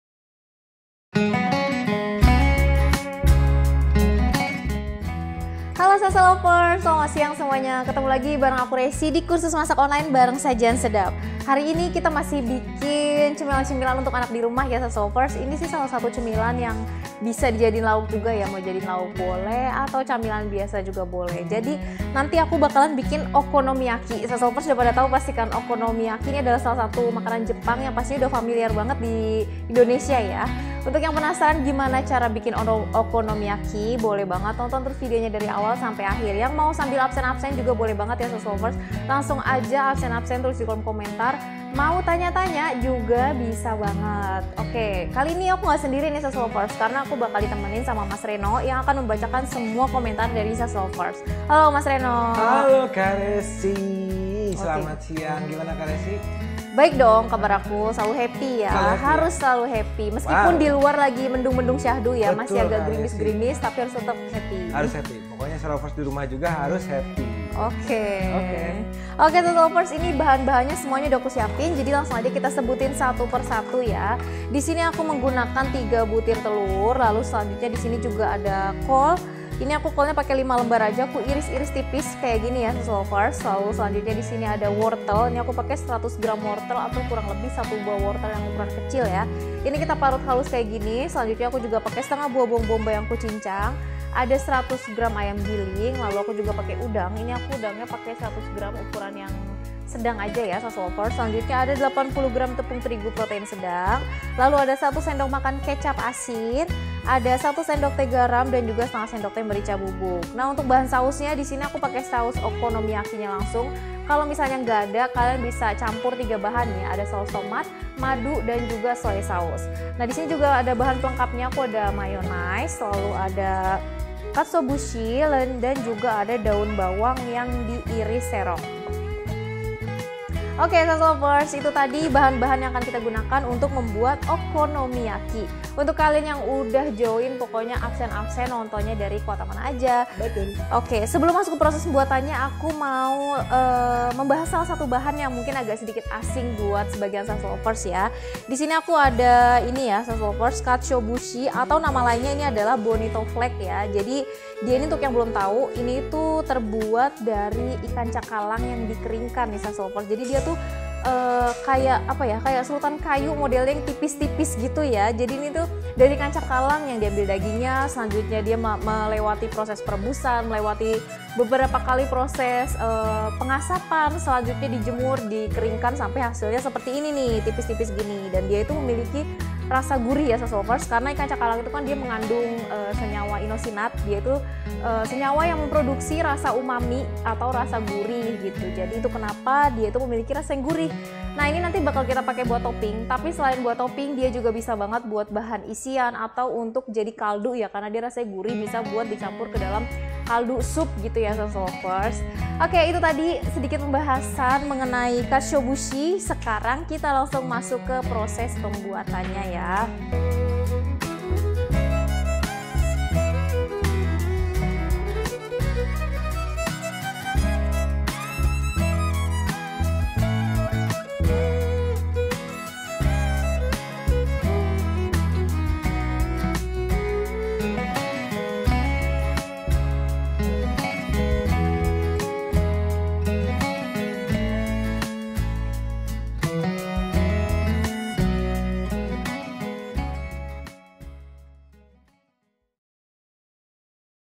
BAHAN OKONOMIYAKI, SIMPEL DAN ENAK BANGET RASANYA!
Mau buat okonomiyaki sendiri di rumah? Bikinnya gampang banget nih, Bun. Yuk, coba resepnya di rumah. Si kecil pasti suka!

OKONOMIYAKI
Bahan:
3 btr telur
5 lbr kol, iris halus
100 gr wortel, iris korek api halus
½ bh bawang bombay, cincang kasar
100 gr ayam giling
100 gr udang ukuran sedang, kupas, kerat punggung\
80 gr tepung terigu protein sedang
1 sdm kecap asin
1 sdt garam
1/2 sdt merica bubuk
Bahan saus;
3 sdm saus tomat
1 sdm maadu
1 sdm soy sc
Bahan pelengkap:
Mayones
Katsuobushi bonito kering
Daun bawang, iris serong

Cara membuat:
1. Saus, campur semua bahan saus jadi 1. Sisihkan.
2. Campur tepung terigu, kol, wortel, dan bawang bombay. Aduk rata.
3. Masukkan telur, kecap asin, garam, dan merica bubuk. Aduk rata.
4. Panaskan pan yang dioles tipis minyak.
5. Tuang adonan. Bentuk bulat pipih diameter 16 cm. tata udang di atasnya.
Biarkan berkulit. Balik dan biarkan matang.
6. Oles dengan saus, coret dengan mayones tipis dan tarik bergantian
dengan tusuk gigi. Sajikan bersama katsuobushi dan daun bawang. (Rs)

Produser Eksekutif: Intan Yusan Septiani
Penulis Resep: Resi
Penulis Naskah: Resi
Peraga: Resi
Fotografer: Osyad
Videografer: Osyad
Video Editor: Osyad